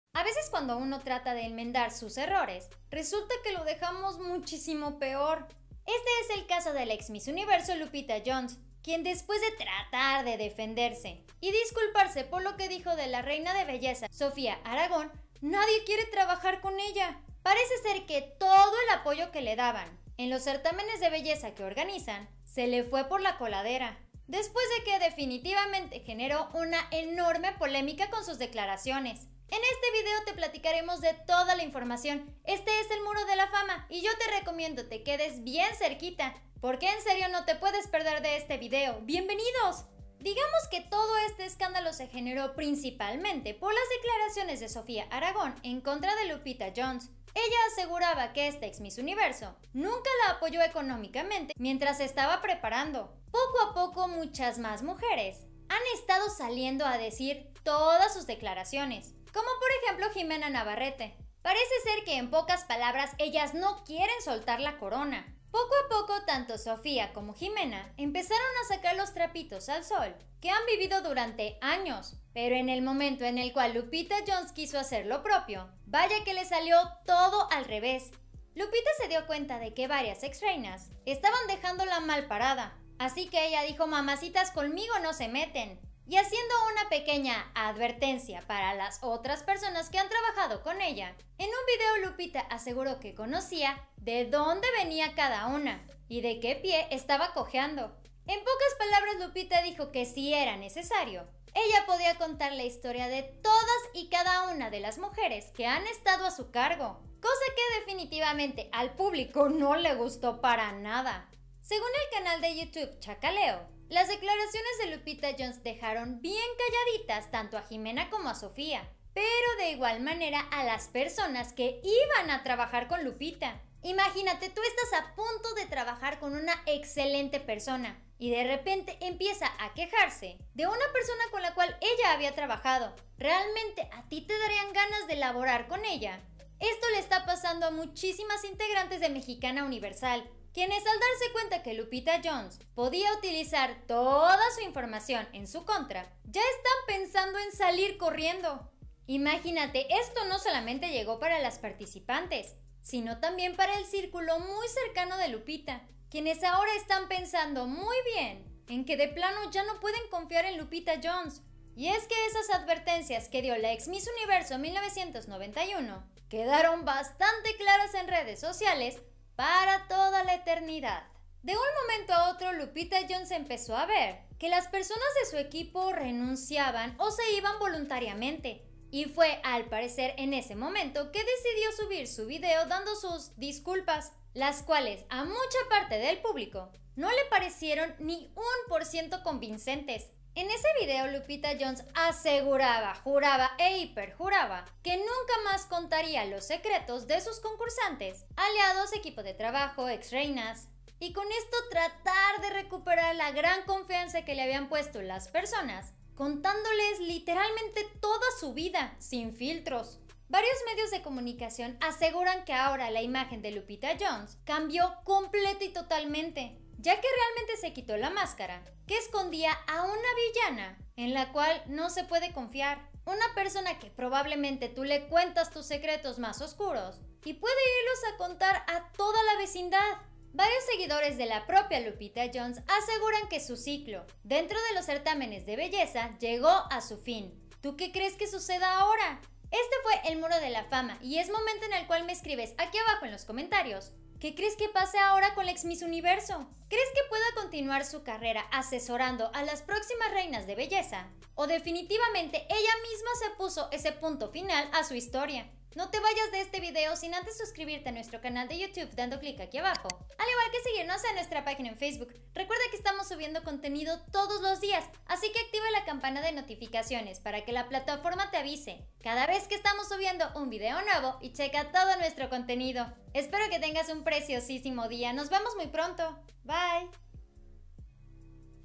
🔴 💥 Asi Es Como 𝗟𝘂𝗽𝗶𝘁𝗮 𝗝𝗼𝗻𝗲𝘀 SE HUNDE y Hace 𝗦𝗨 𝗣𝗥𝗢𝗣𝗜𝗔 𝗧𝗨𝗠𝗕𝗔 😱
Lupita Jones arruina su carrera por esto...

😡 🔥 ELEAZAR GOMEZ 𝗖𝗼𝗺𝗽𝗮𝗿𝗲𝗰𝗲 𝗔𝗻𝘁𝗲 𝗝𝘂𝗲𝘇 Y Esto Fue Lo Que Dijo.😡 🔥
🔴📢Ferdinando Valencia 𝗥𝗼𝗺𝗽𝗲 𝗘𝗹 𝗦𝗶𝗹𝗲𝗻𝗰𝗶𝗼 Y Hara Gran Denuncia 𝗘𝗻 𝗡𝗼𝗺𝗯𝗿𝗲 𝗗𝗲 𝗦𝘂 𝗛𝗶𝗷𝗼 🔴📢